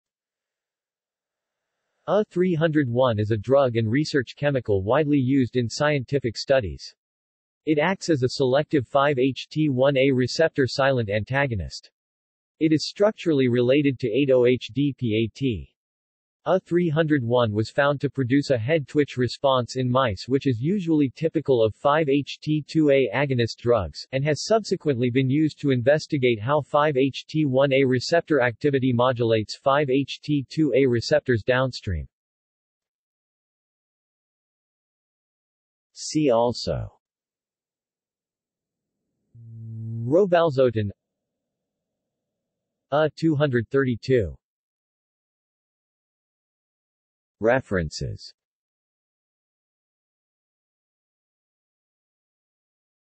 UH-301 | Wikipedia audio article
This is an audio version of the Wikipedia Article:
UH-301

SUMMARY
UH-301 is a drug and research chemical widely used in scientific studies. It acts as a selective 5-HT1A receptor silent antagonist. It is structurally related to 8-OH-DPAT. UH-301 was found to produce a head-twitch response in mice which is usually typical of 5-HT2A agonist drugs, and has subsequently been used to investigate how 5-HT1A receptor activity modulates 5-HT2A receptors downstream.